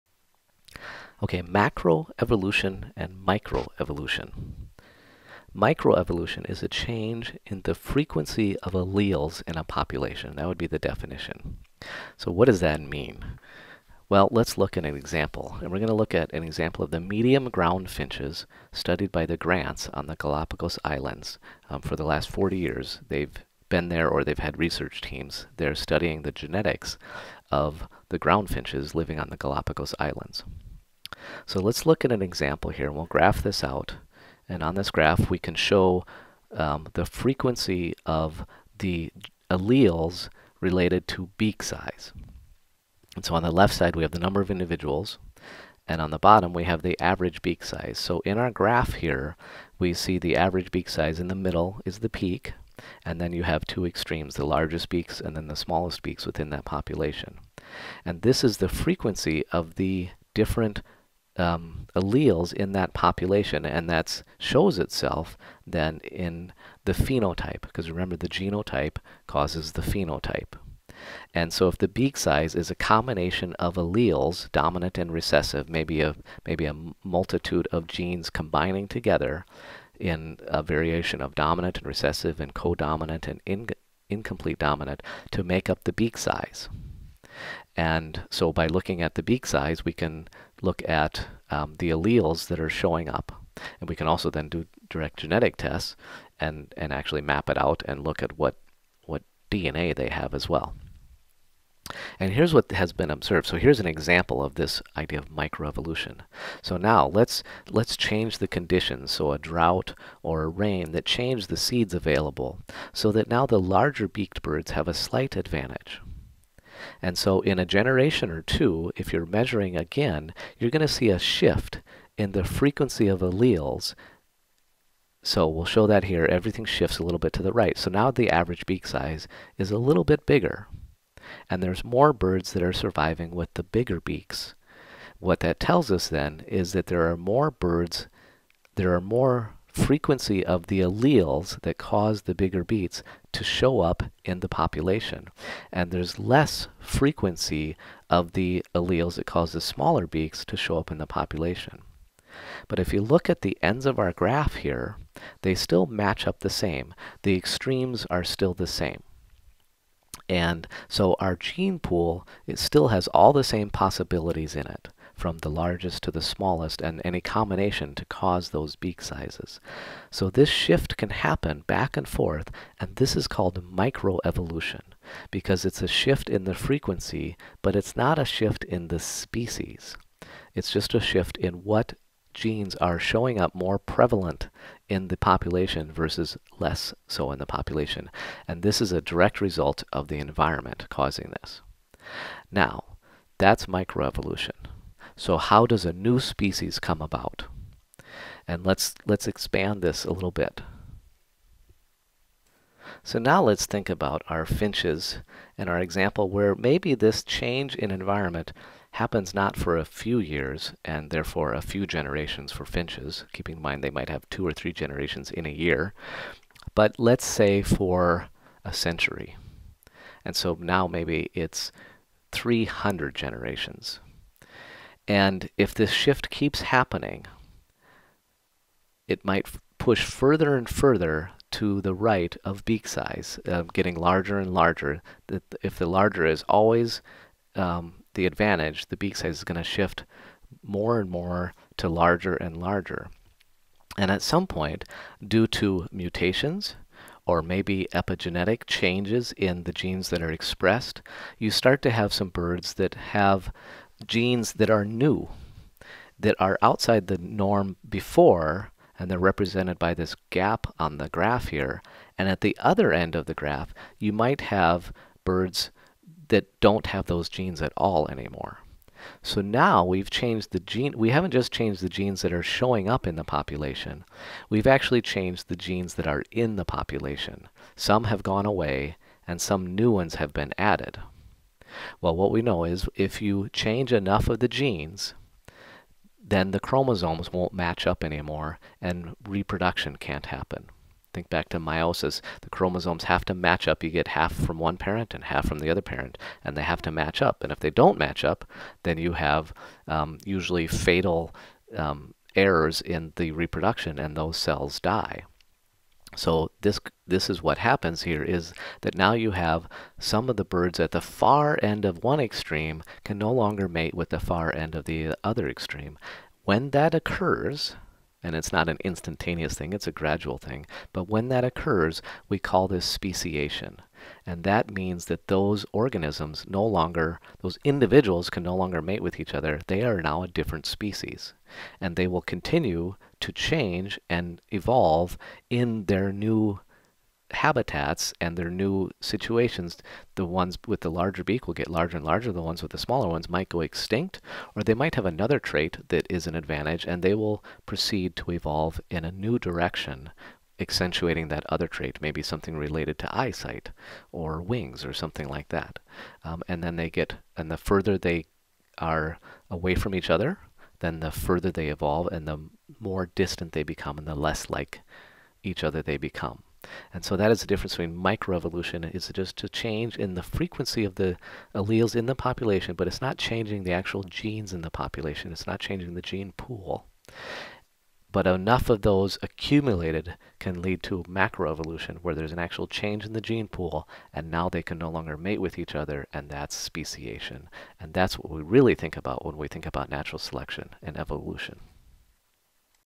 Microevolution and Macroevolution
This describes the difference between microevolution and macroevolution. This video is a supplement to the book, "Ecological Identity: Finding Your Place in a Biological World" and the biology curriculum, "Biology: Exploring Your Role in Biodiversity."